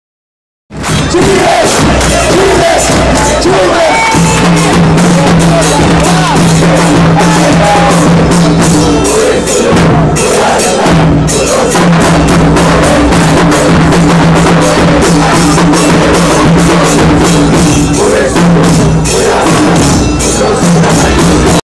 Torcida organizada do Club The Strongest (Tigres da Bolívia)
Jogo de futebol contra o time Bolivar em maio de 2012.